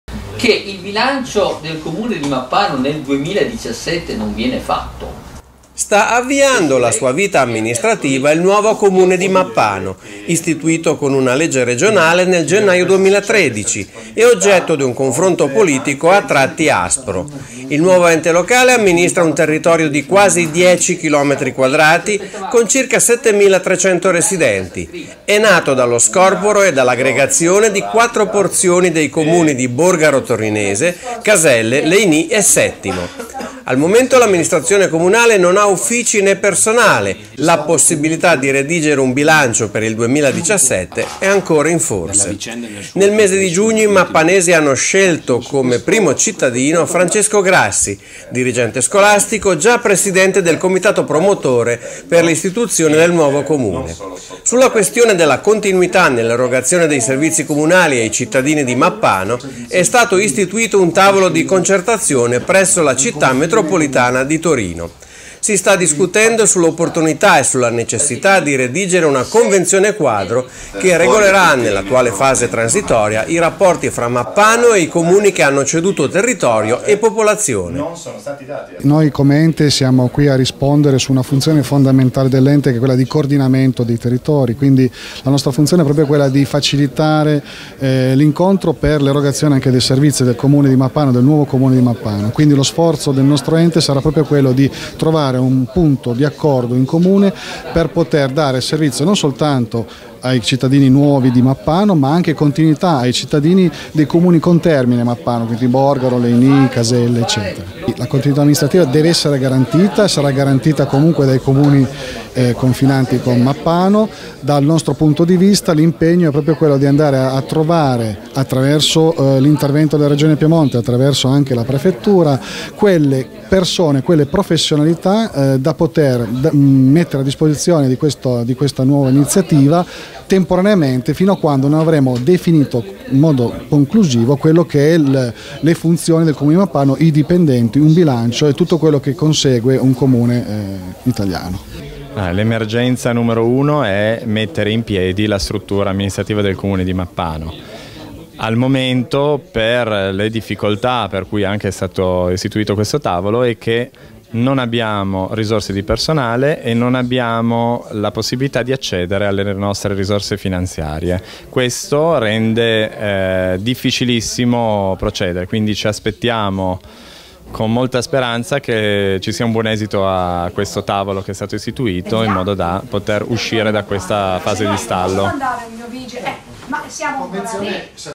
Un tavolo di concertazione per Mappano
La Città metropolitana di Torino ha istituito un tavolo di concertazione per la continuazione dei servizi comunali ai mappanesi